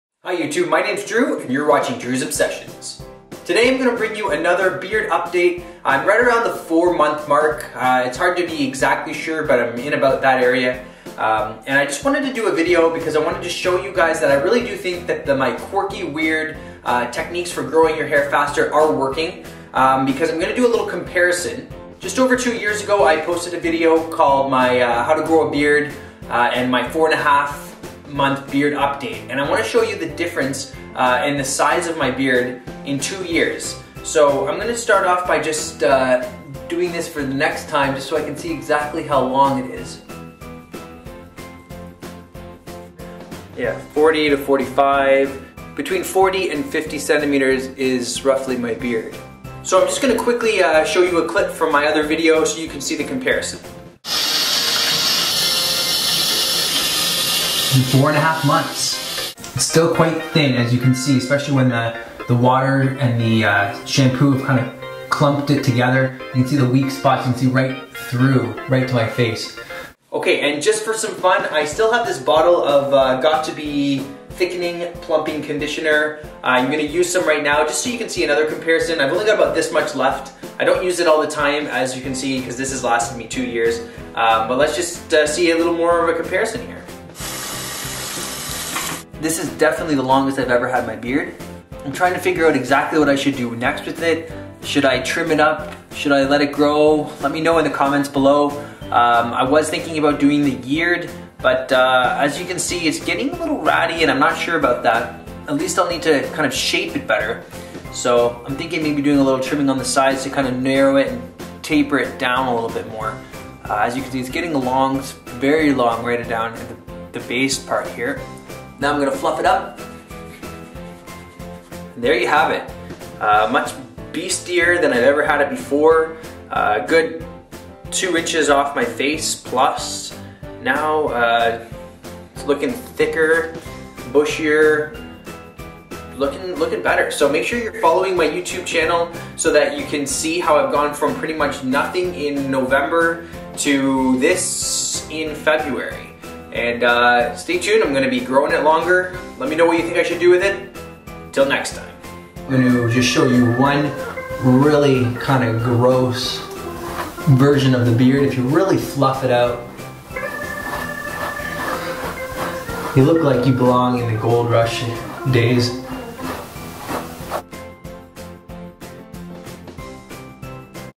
HOW to GROW A Beard: BEARD Update 4 months "PROOF IT's THICKER"
In this video I compare my beard now and how it looked 2 years ago. I think you will be amazed to see the difference.